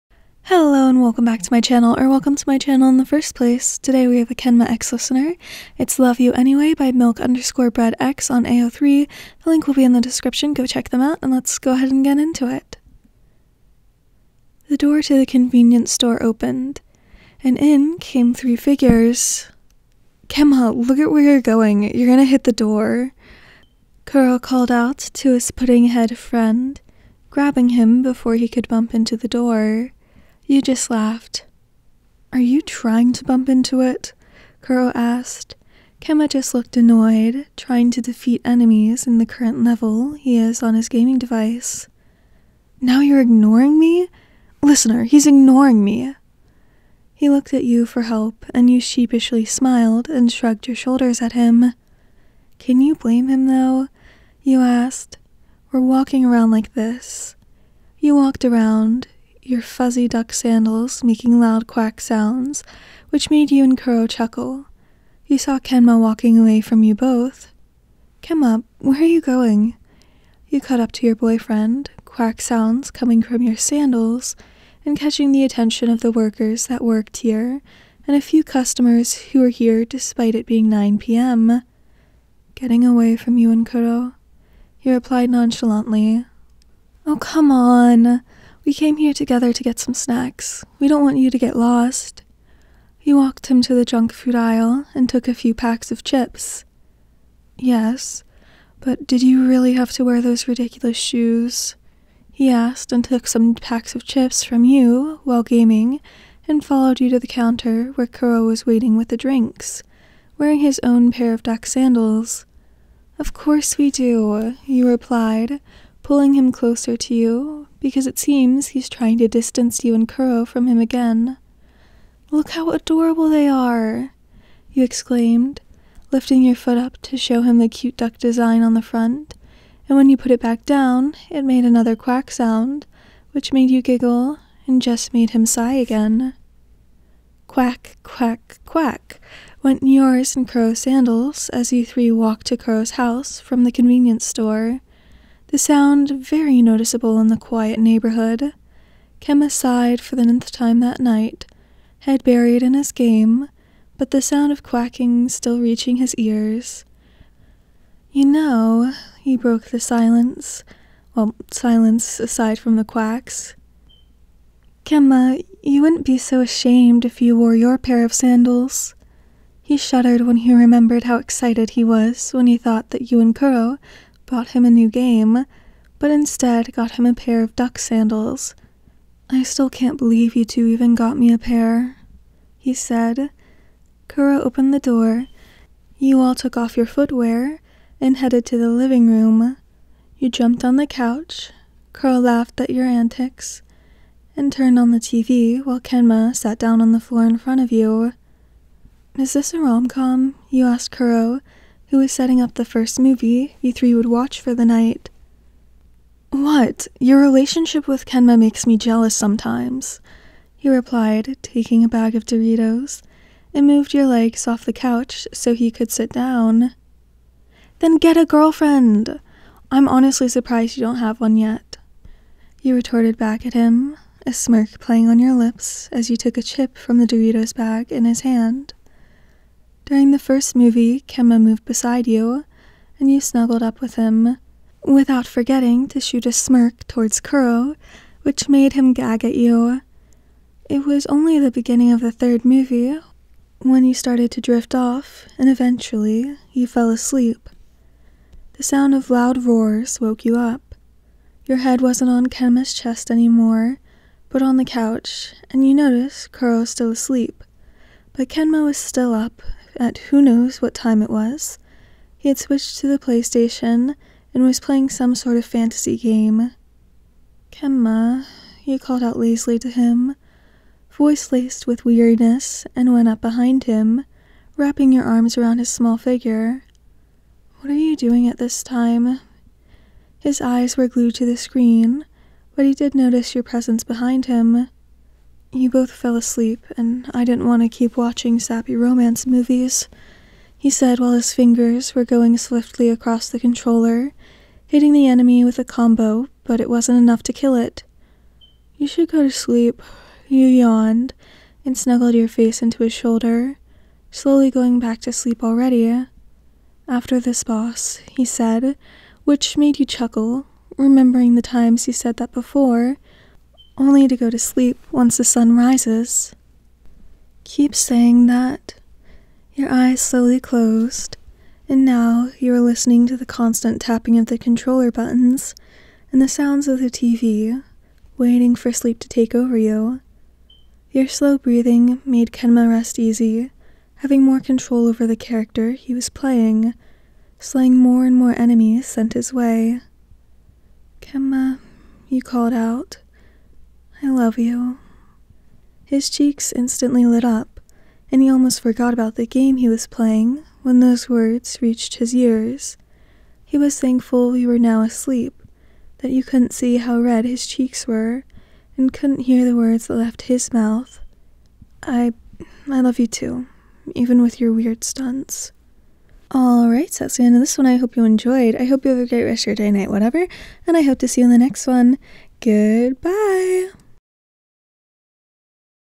"Love You Anyway" | No BGE | Kenma Kozume x Listener {Haikyuu Fanfic Reading}
Summary: Complete Fluff with some Crack!
You, Kenma and Kuroo decided to go to the convenience store. Kenma is happy you guys decide to go there at NIGHT.
Why?